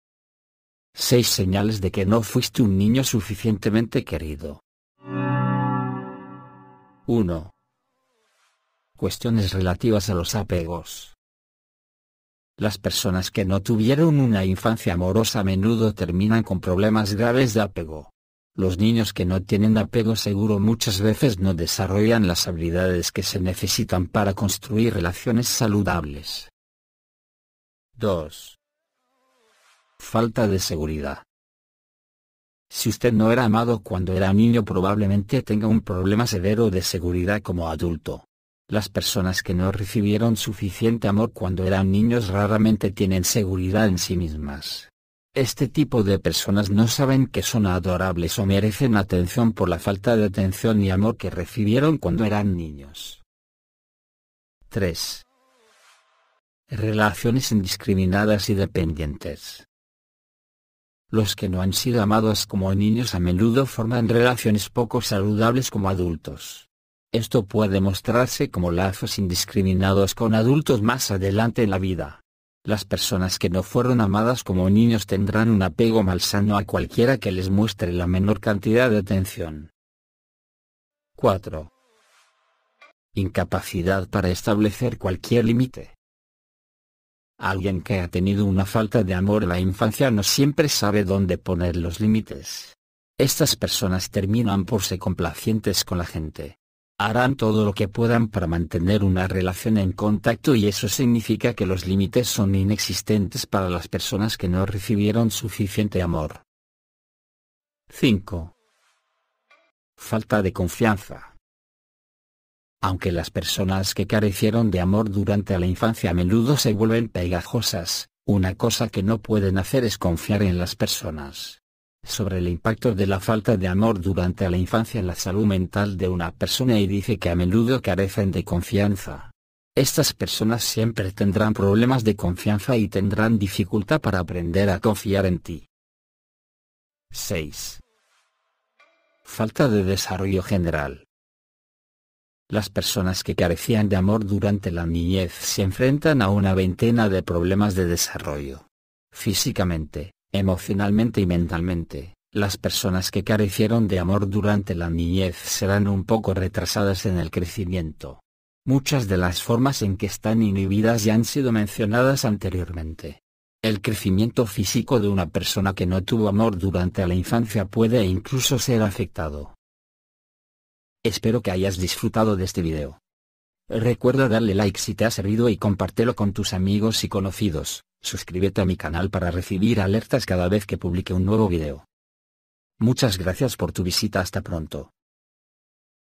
6 Señales de que no fuiste un niño querido ✔️Psicología
Hay seis señales cruciales de que alguien no era lo suficientemente querido como niño que cada persona debería prestar atención. Psicologia Visual

... más información en el video ...